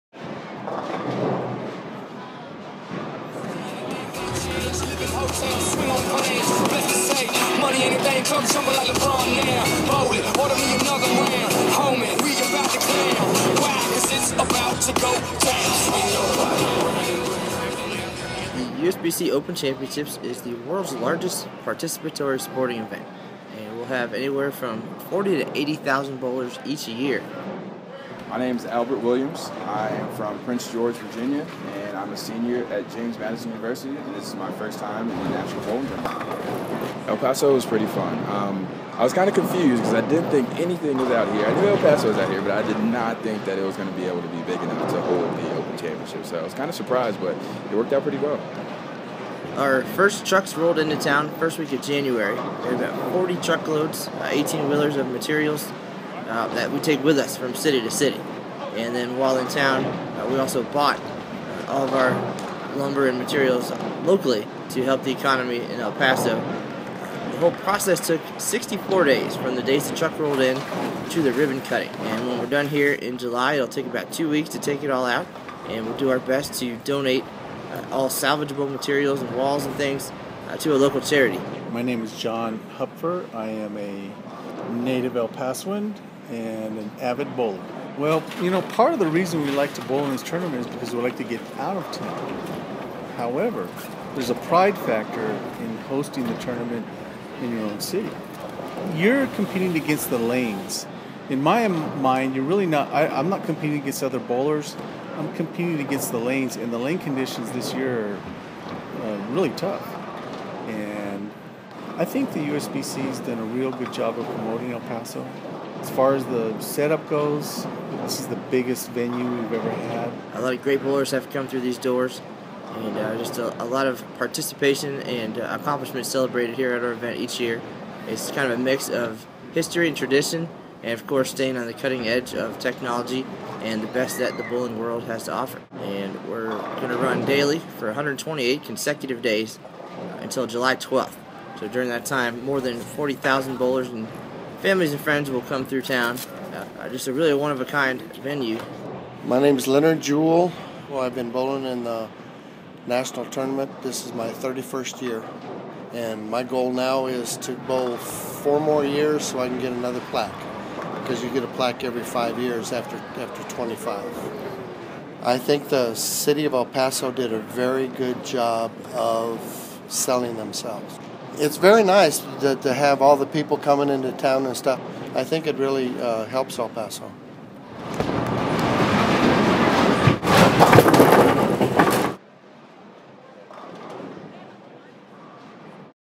2015 USBC Open Championship in El Paso
Judson F. Williams Convention Center in Downtown El Paso was transformed into a 62-lane bowling complex for the 2015 USBC Open Championship.